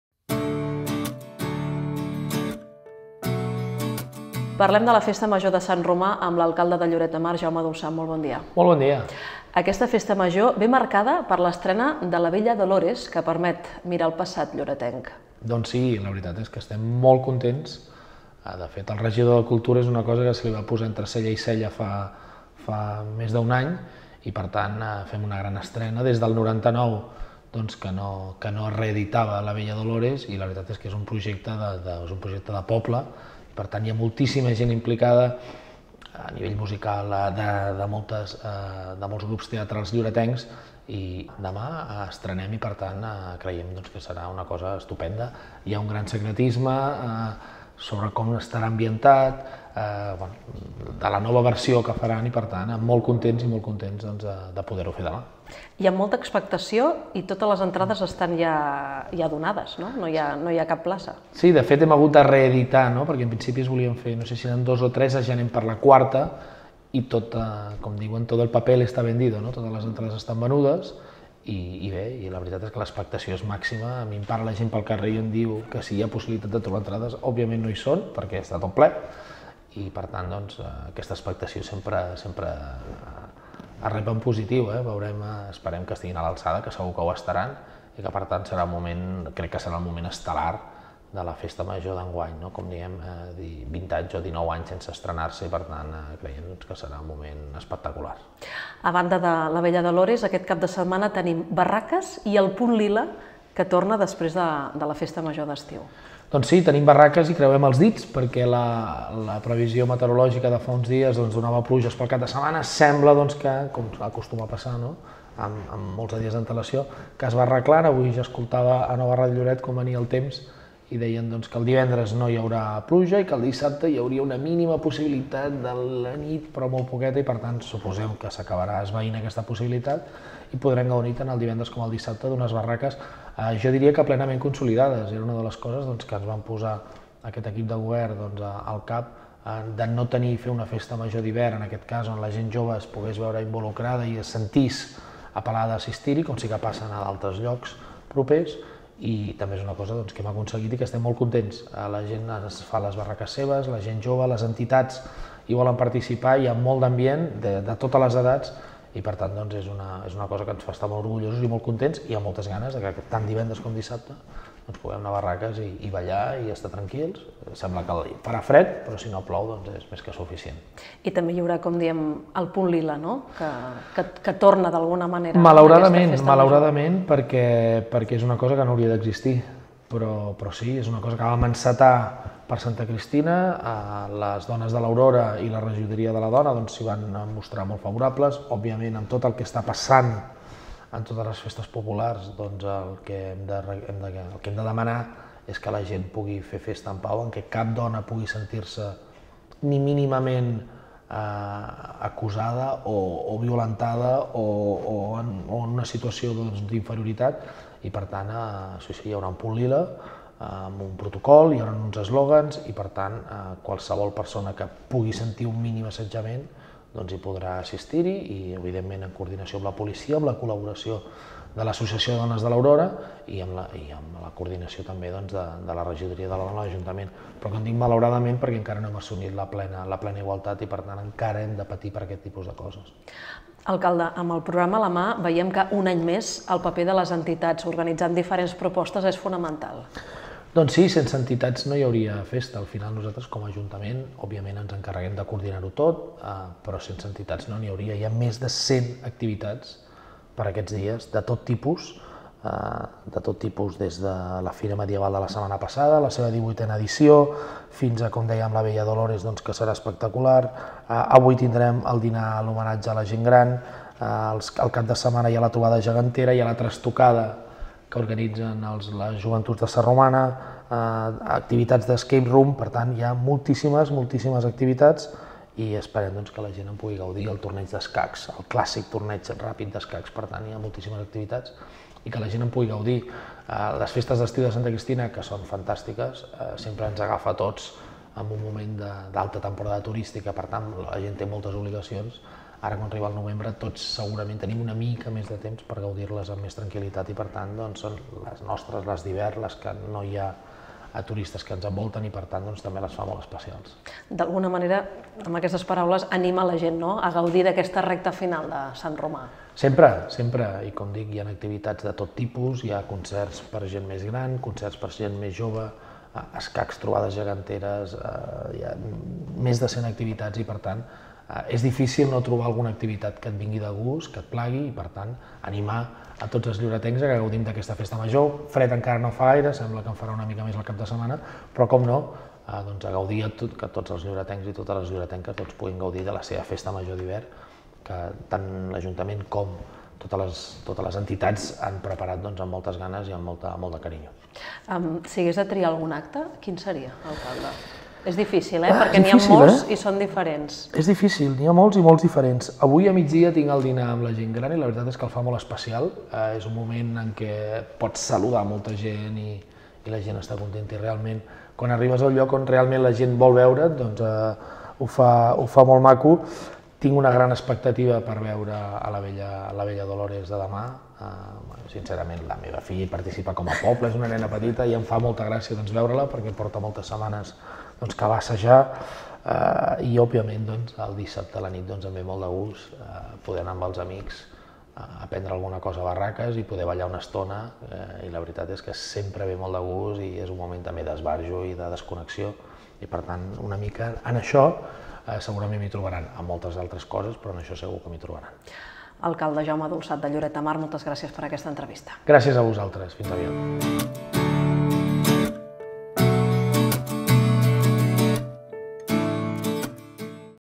ENTREVISTA JAUME DULSAT
Aquest dijous arriba el pregó de Sant Romà i l'estrena de 'LA BELLA DOLORES'. A punt d'encarar la recta final de la festa major, entrevistem l'alcalde de Lloret de Mar, Jaume Dulsat.